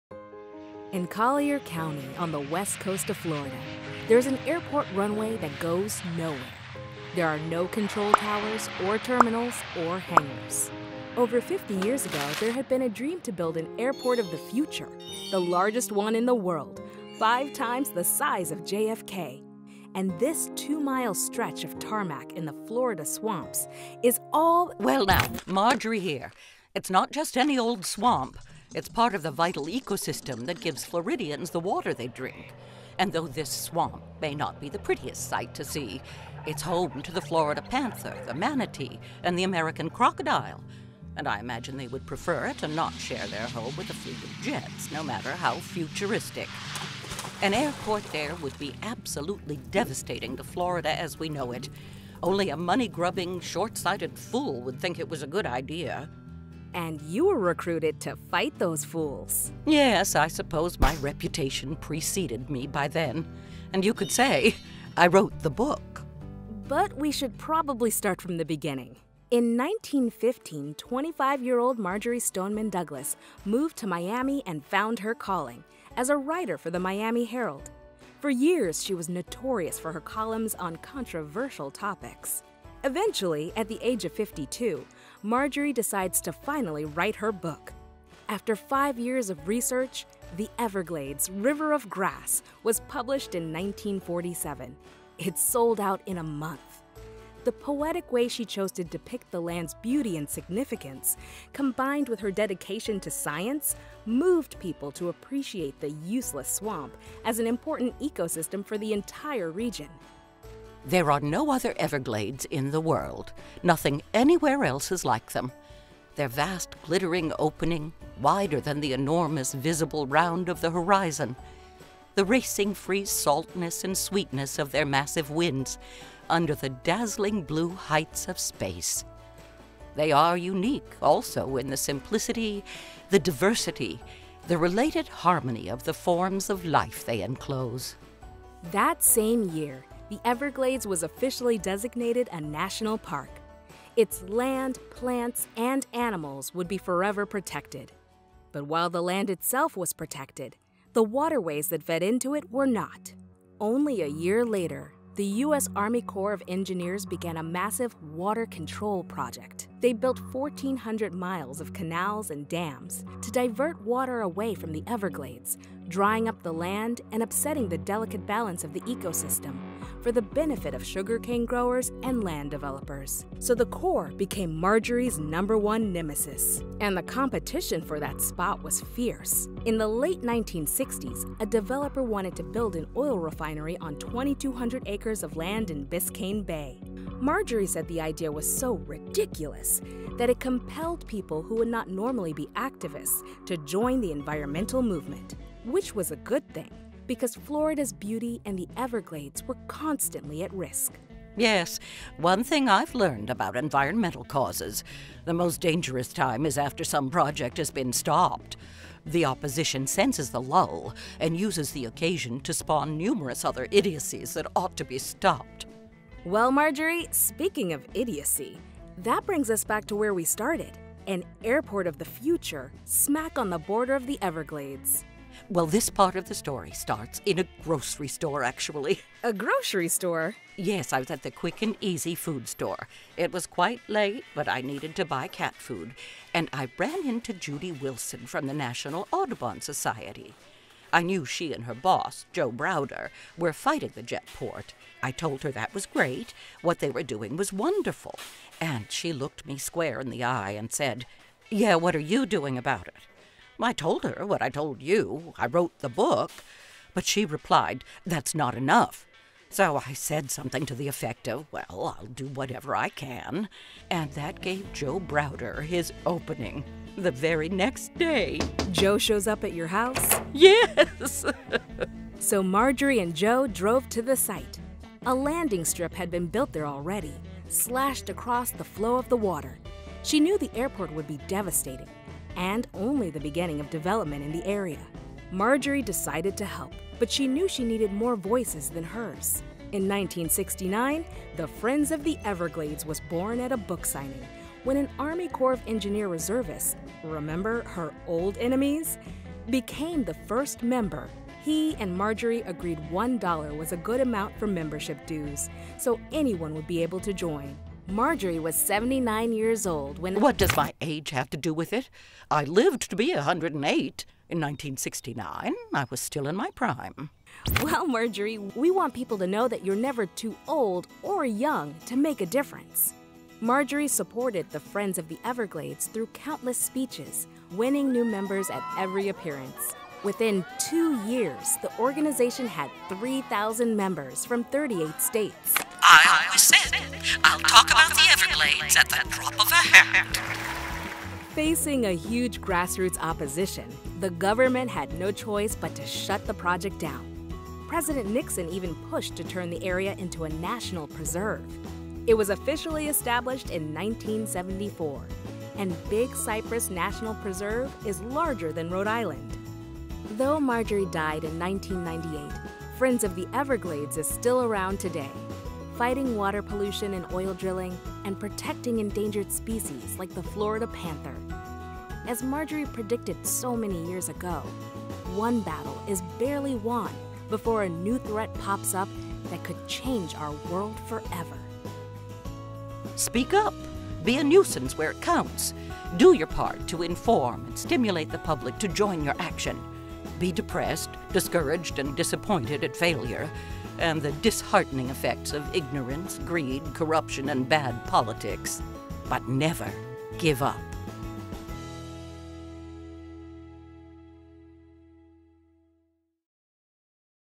HULU | Collaboration with BWA Studios for Women’s History Month
This animation was created in collaboration with BWA Studios for Women’s History Month to discuss the contributions of Majorie Stoneman Douglas and it ran as a social spot.

Animation by:
Reel Black Studios

We are a Brooklyn-based, black-owned, dope ass animation studio with a passion in working with brands to tell a good story.